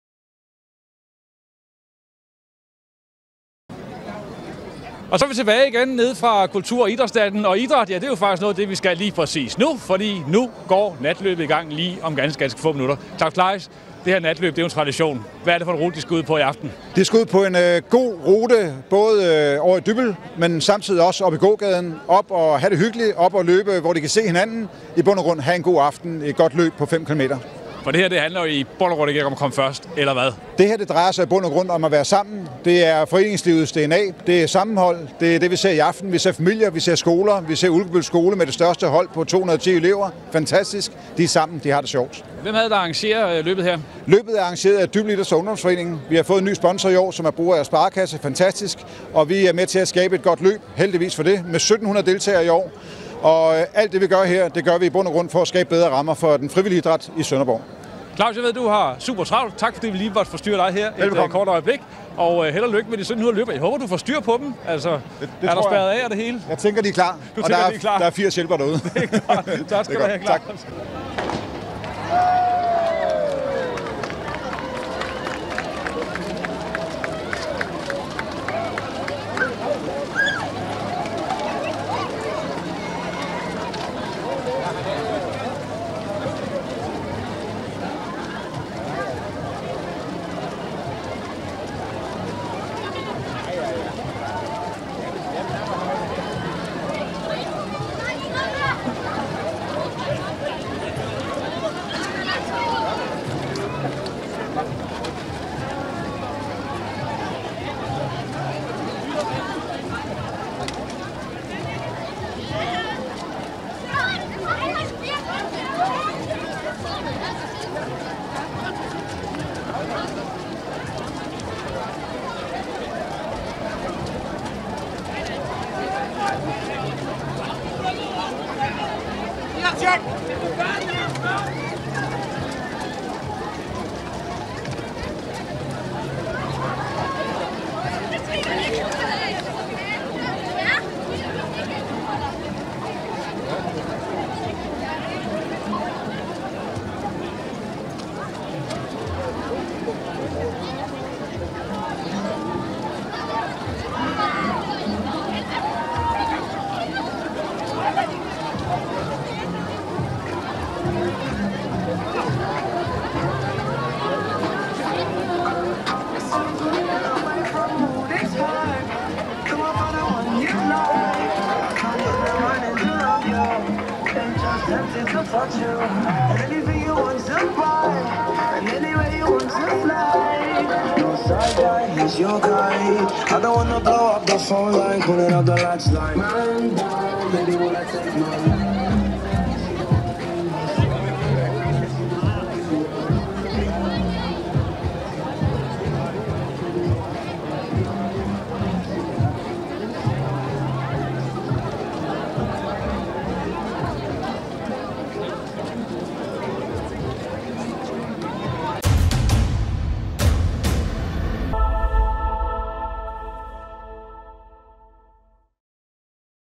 Natløbet starter til Kultur– og Idrætsnatten 2018 Sønderborg
Fredag den 24. august var der Kultur- og Idrætsnat i Sønderborg og SønderborgNYT sendte direkte fra Havnefronten og SønderborgNYT var med da natløbet blev skudt i gang.

Reporter/redigering: Michael Kjær
Foto/Live-operatør: Ole Kæhler

Dette er en redigeret udgave af live-sendingen.